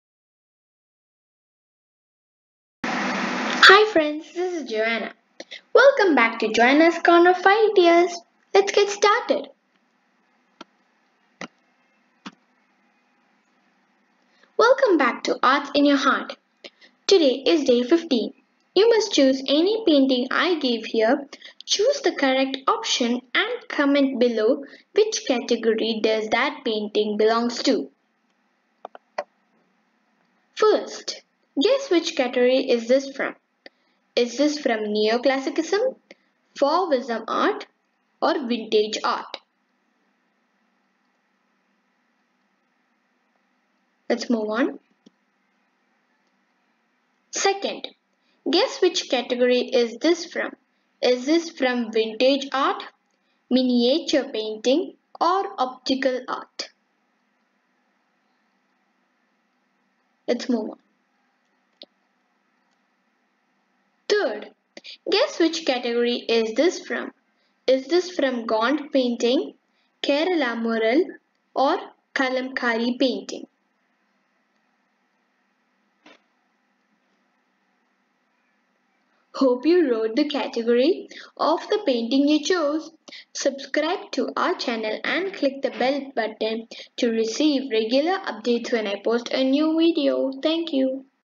20 Days Art in your hearts Day 15 | Joanna's corner of ideas!
Hi friends! Today is day 15 of our 20 days Arts in your hearts. I have  given 3 pictures and on the side of it I have given 3 different options, you must choose a picture, and then choose the correct option and comment below which picture you chose and the option you chose.